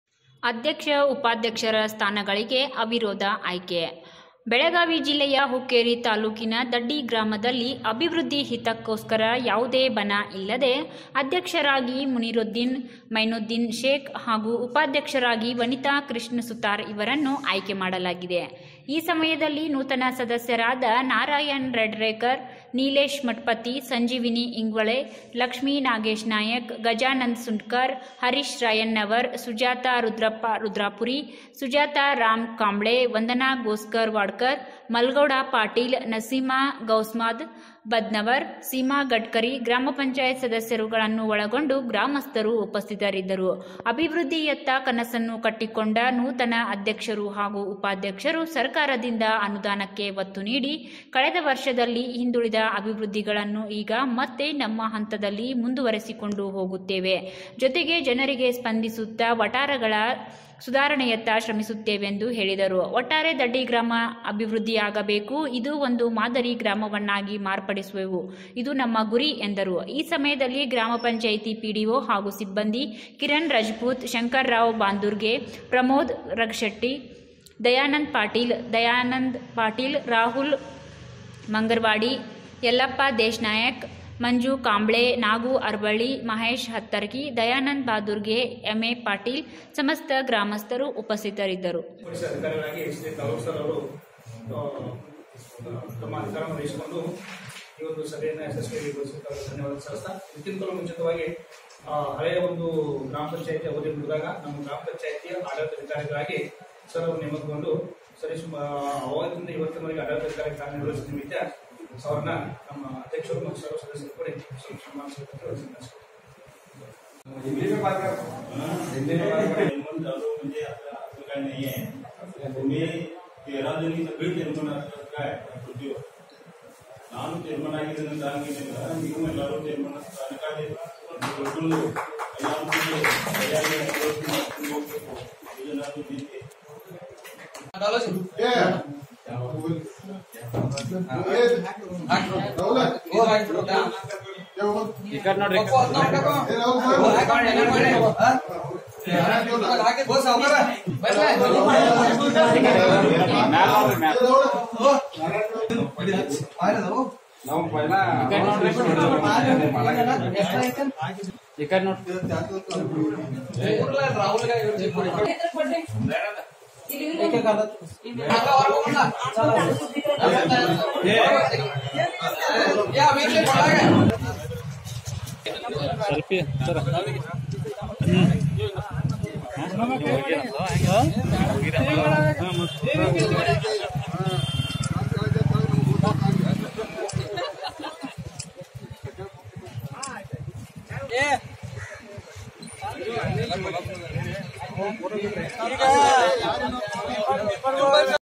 ದಡ್ಡಿ ಗ್ರಾಮ ಪಂಚಾಯಿತಿಗೆ ಅಧ್ಯಕ್ಷ-ಉಪಾಧ್ಯಕ್ಷರ ಸ್ಥಾನಗಳಿಗೆ ಅವಿರೋದ ಆಯ್ಕೆ || karnataka supar fast news
Karnataka Supar Fast NEWS 24X7